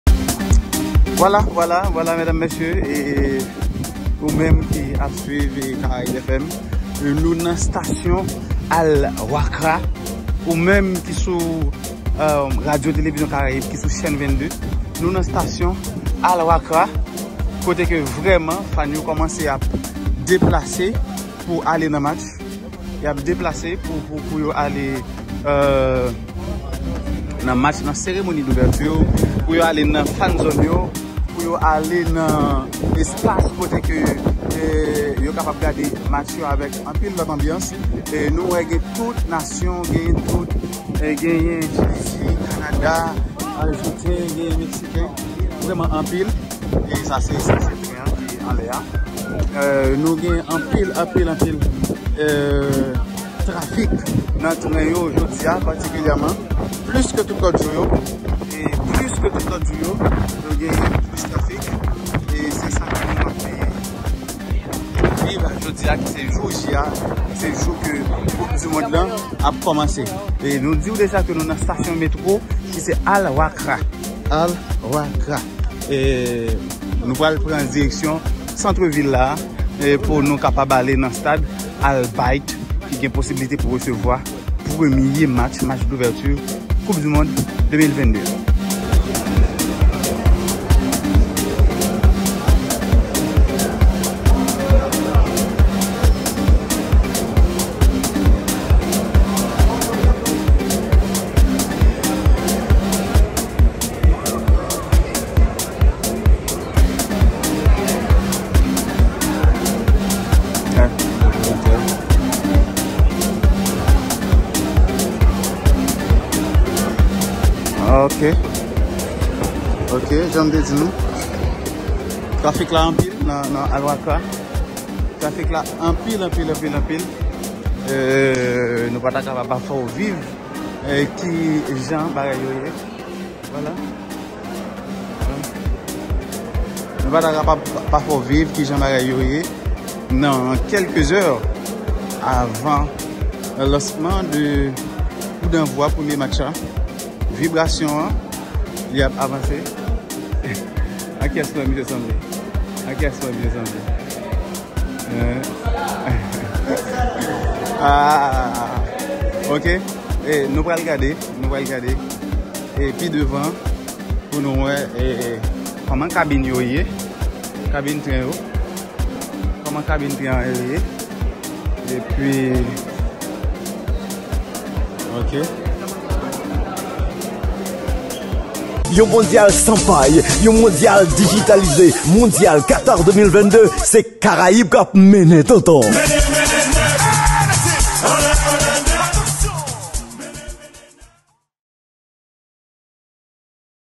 A quelques heures de la cérémonie inaugurale de la 22e édition de la coupe du monde
A quelques heures de la cérémonie inaugurale de la 22e édition de la coupe du monde, La RTVC, via notre envoyé spécial au Qatar, Lutherson Léon, vous invite à vivre l’ambiance qui règne à l’extérieur du stade où des supporters fluent déjà l’enceinte de Al Bayt Stadium.